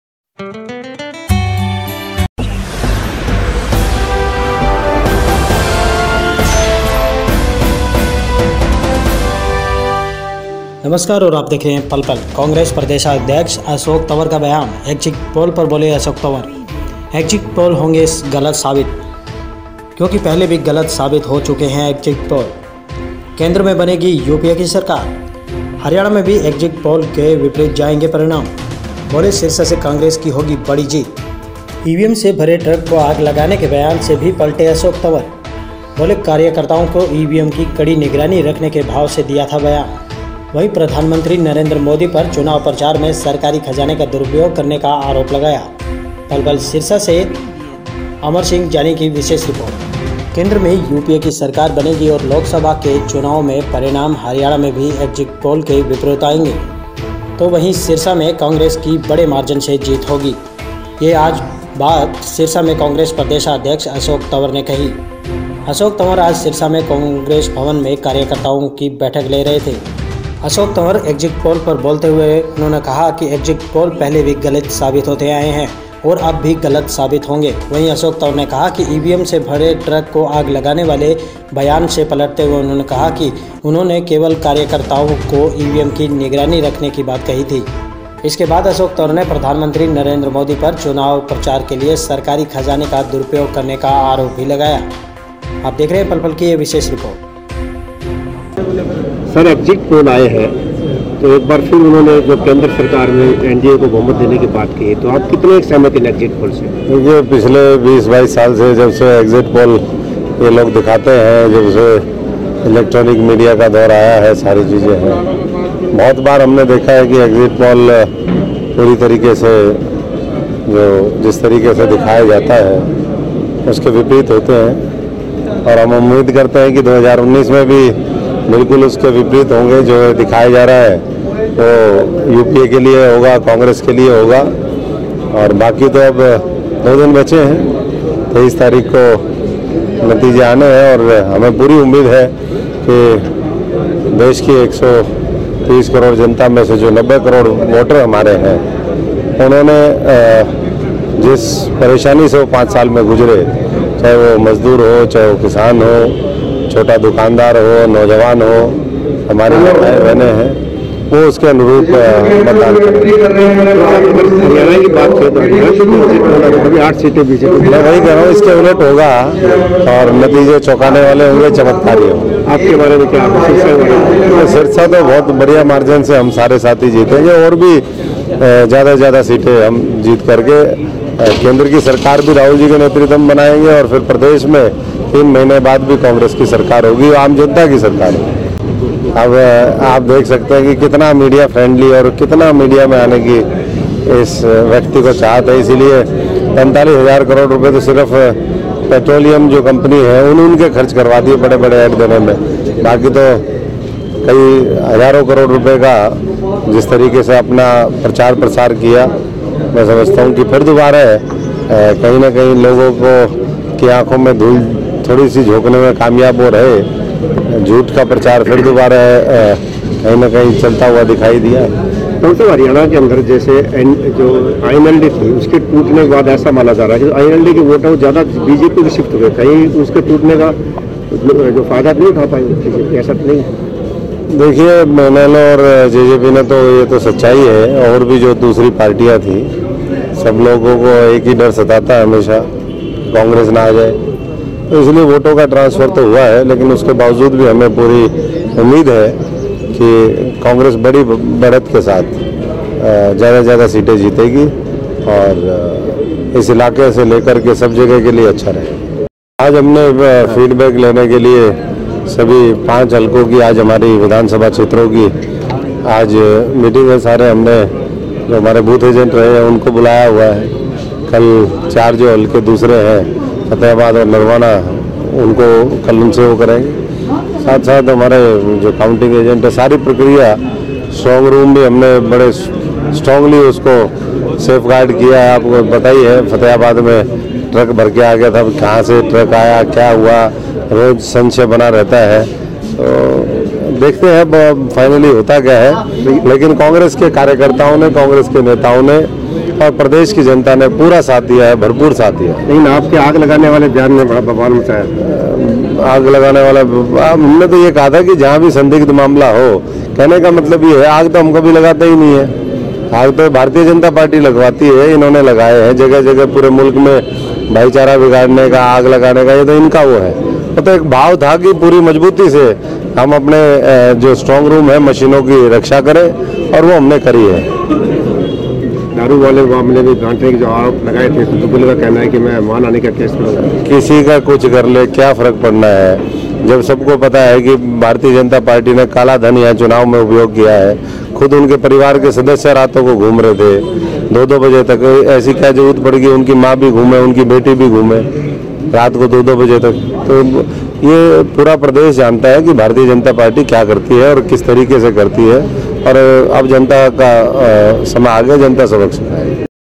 पोल होंगे गलत साबित-यूपीए की बनेगी सरकार-अशोक तंवर का बयान
केंद्र में यूपीए की सरकार बनेगी और लोकसभा के चुनावो के परिणाम हरियाणा में भी एग्जिट पोल के विपरीत आएंगे। तो वही सिरसा से कांग्रेस की बड़े मार्जिन से जीत होगी। यह बात आज सिरसा में कांग्रेस प्रदेश अध्यक्ष अशोक तंवर ने कही। अशोक तंवर आज सिरसा के कांग्रेस भवन में कार्यकर्ताओ की बैठक ले रहे थे।
अशोक तंवर एग्जिट पोल पर बोलते हुए कहा कि एग्जिट पोल पहले भी गलत  साबित होते आये है और अब भी गलत साबित होंगे। वही अशोक तंवर ने ईवीएम से भरे ट्रक में आग लगाने वाले बयान से पलटते हुए कहा कि उन्होंने तो सिर्फ कार्यकर्ताओ को ईवीएम पर कड़ी निगरानी रखने की बात कही थी। वही अशोक तंवर ने प्रधानमंत्री मोदी पर चुनाव प्रचार में सरकारी खजाने के दुरप्रयोग करने का भी आरोप लगाया।